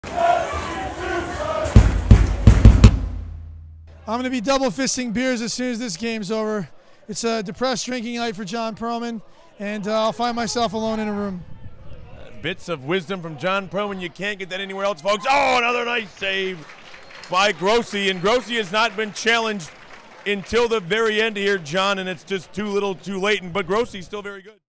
CHANCE: Ornstil shoots from distance, deflected
In the 90+5 minute, FC Tucson takes it's last real chance in the Western Conference Finals with Ornstil firing a shot from a distance but it ends up being knocked away by the Kitsap Pumas goal keeper. Final: 0-3